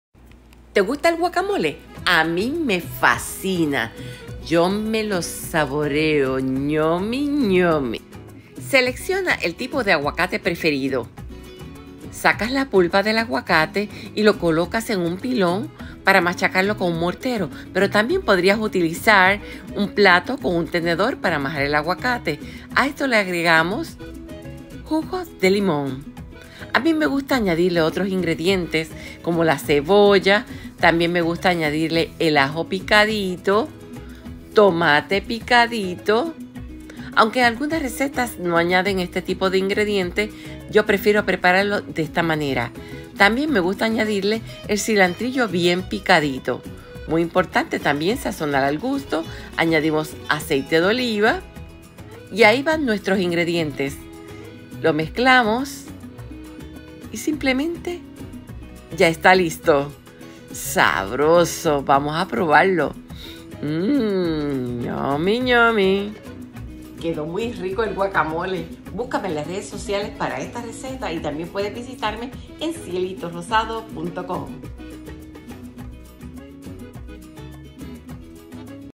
GUACAMOLE
El Guacamole 🥑 Esa salsa fría fácil de preparar y que puedes servir con nachos, servir como acompañamiento con unos sabrosos taquitos o servirlo sobre unas tostadas de pan!! simplemente sabroso! aquí mi versión! mi receta! que la disfruten!